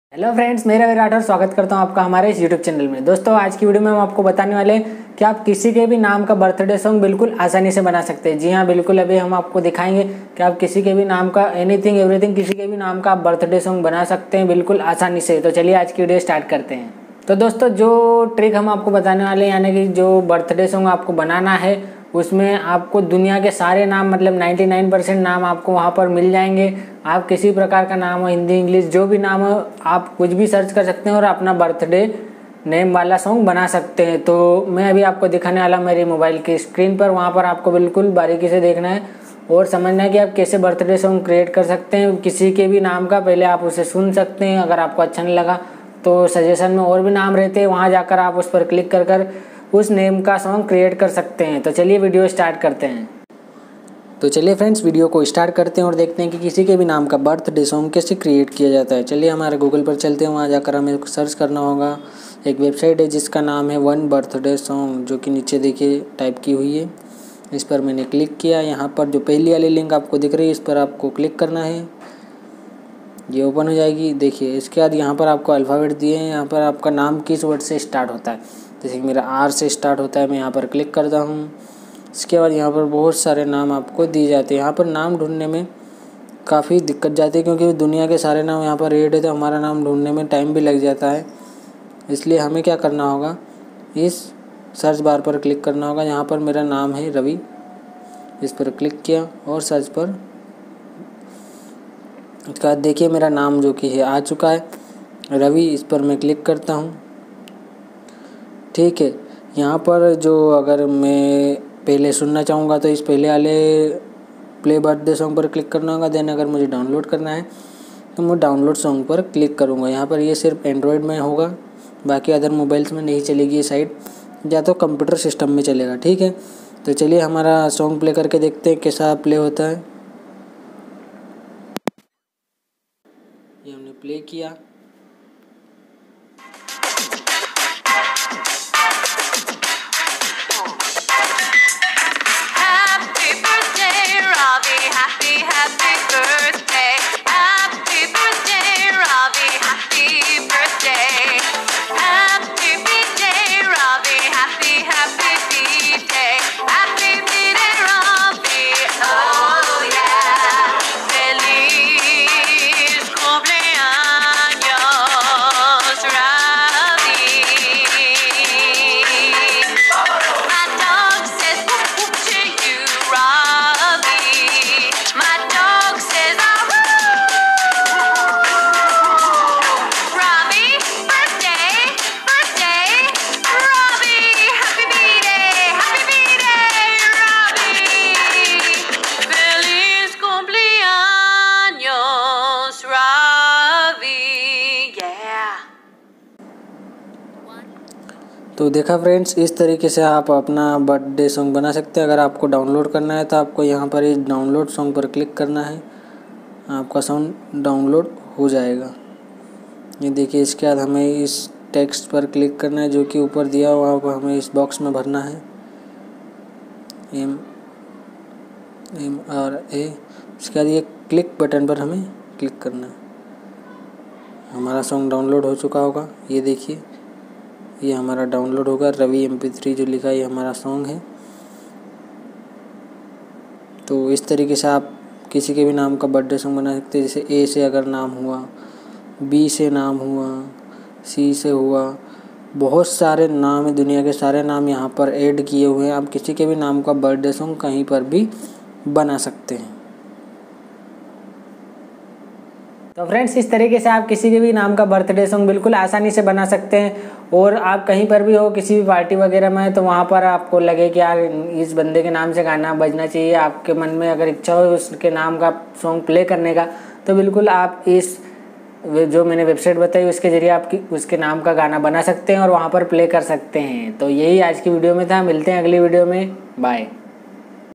Kisi Ke Bhi Naam Ka Birthday Song Kaise Banaye . VID 51
Hii Friends
Dosto Aaj Ki Video Me Ham Aapko Batayenge Ki Aap Kisi ke Bhi Naam Ka Birthday Song Kaise Bana Sakte Hai Or Kaise Use Download Kar Kar Sakte To Aaj Ki Video Me Website ke Jariye Aapko Sari Jankari Batane Wale Hai ,

To Is Video Ko Last Dekhiye Or Samajhiye .

Thanks
Aise hi hamara support kijiye friends